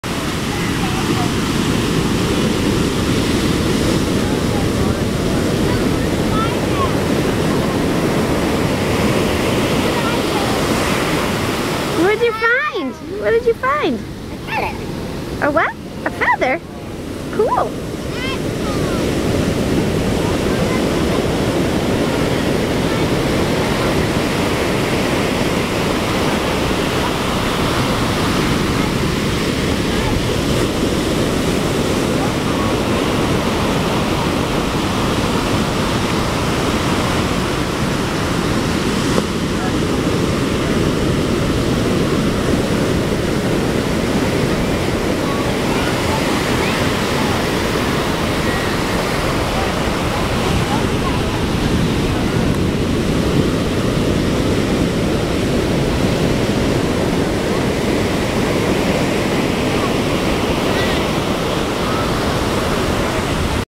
Boogie 'n Bill
April, Mark, Mike and Pete braving the swells of Hurricane Bill